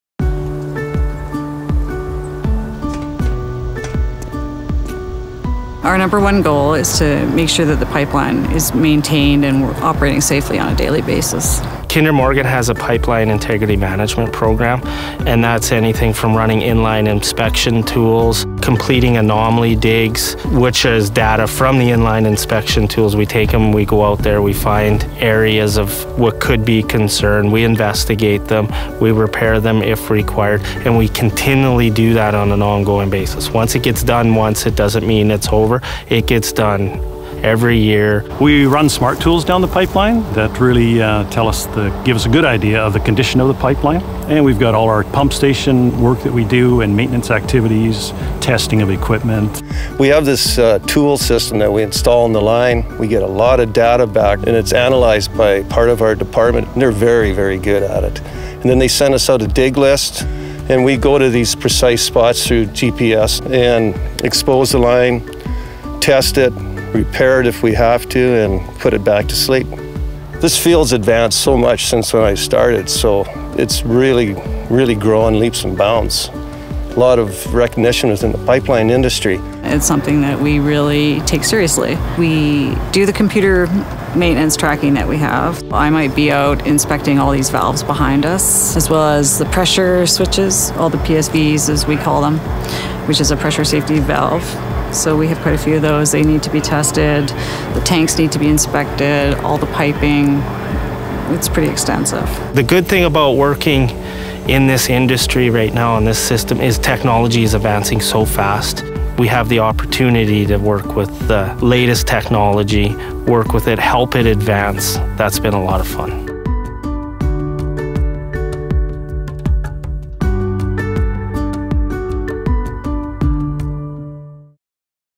The People Behind The Pipeline - Maintenance and integrity
The people responsible for maintaining the integrity of the pipeline discuss the many inspection and maintenance activities and innovations that go in to it.

Les personnes chargées de maintenir l'intégrité du pipeline discutent des nombreux travaux d'inspection et de maintenance et des innovations qui y sont liées.